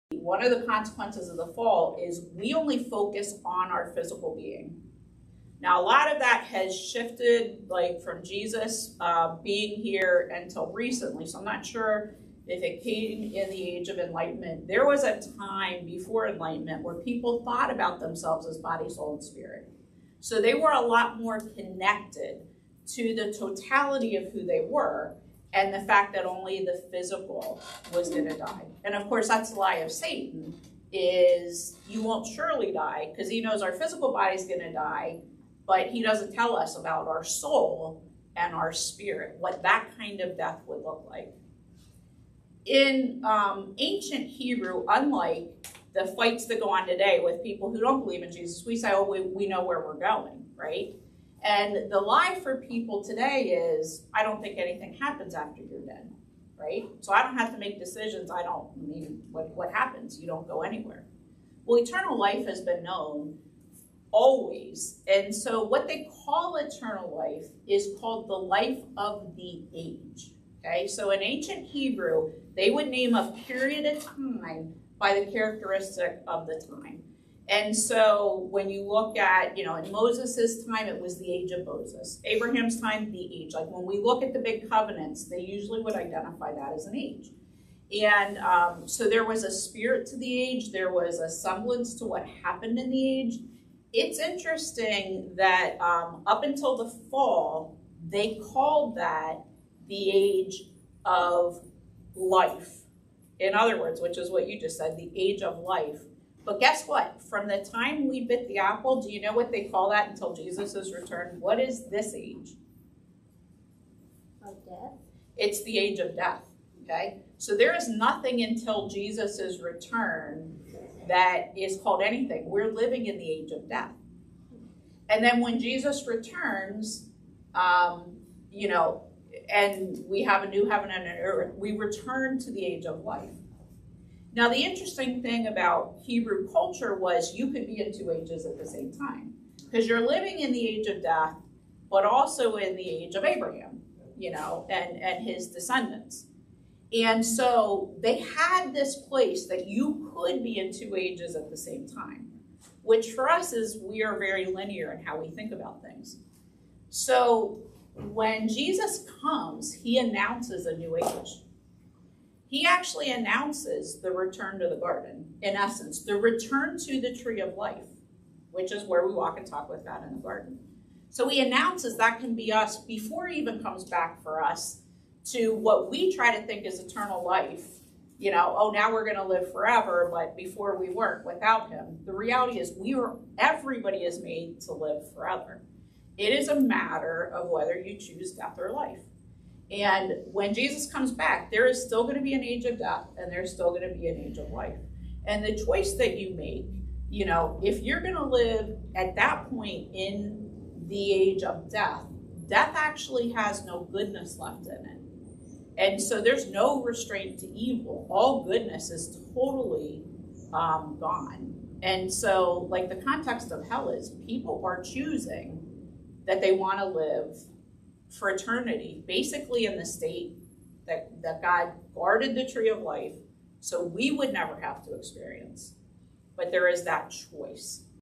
What Does Eternal Life Mean?
Many believe that life ends after death. All life. The Bible says otherwise. Let's take a quick glimpse at Body, Soul and Spirit. What really happens next.

We have a great deal to look forward to as Christ-followers. For all others, the choice is theirs.